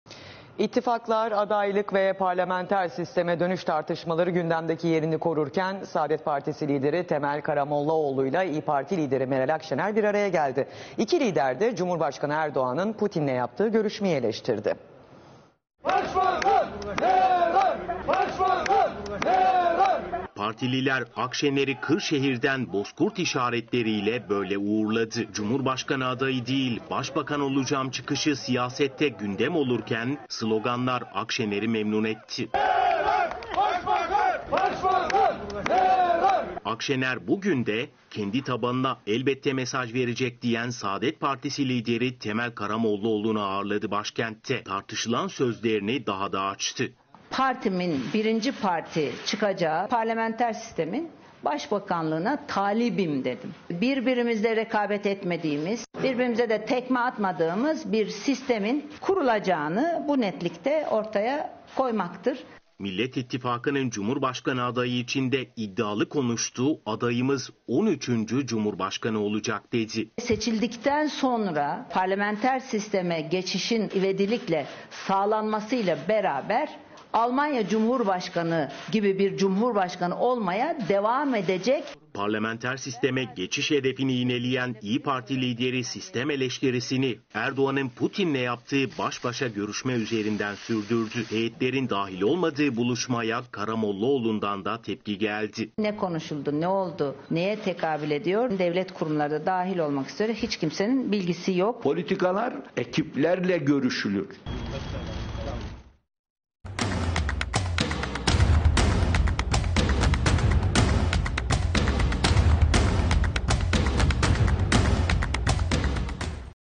Akşener'e yeni slogan
İTTİFAKLAR, ADAYLIK VE PARLAMENTER SİSTEME DÖNÜŞ TARTIŞMALARI GÜNDEMDEKİ YERİNİ KORURKEN SAADET PARTİSİ LİDERİ TEMEL KARAMOLLAOĞLU İLE İYİ PARTİ LİDERİ MERAL AKŞENER BİR ARAYA GELDİ. İKİ LİDER DE, CUMHURBAŞKANI ERDOĞAN'IN, PUTİN İLE YAPTIĞI GÖRÜŞMEYİ ELEŞTİRDİ